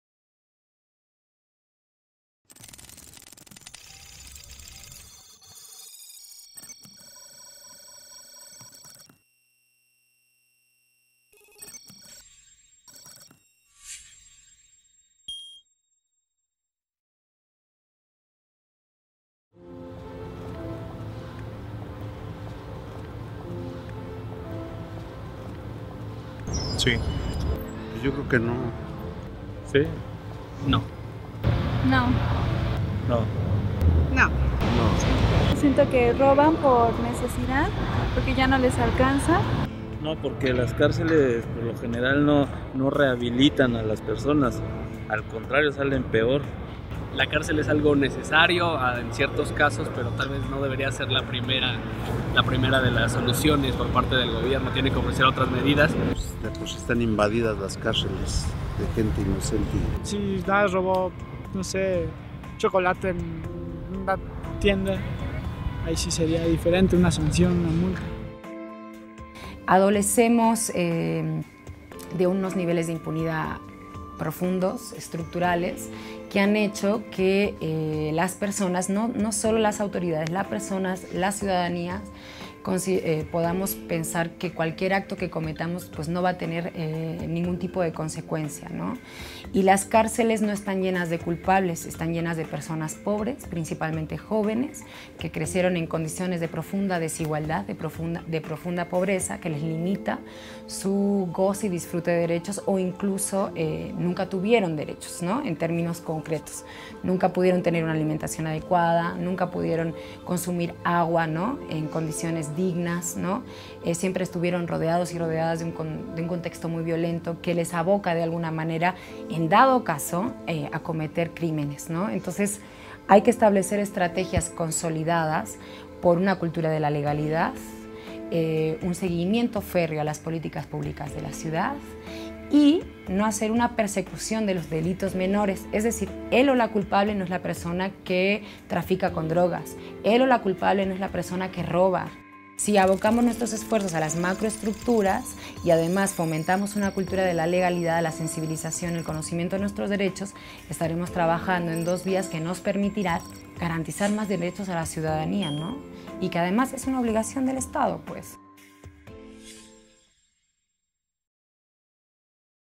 Vlog: ¿Qué hay detrás del delito de robo? ¿Merece cárcel?
"Las cárceles están llenas de gente pobre", explica una especialista. No te lo pierdas, y pensemos juntas y juntos en alternativas a la cárcel para delitos menores.

Twitter.com/DocumentaAC

Facebook.com/DocumentaAC

Instagram.com/Documenta_AC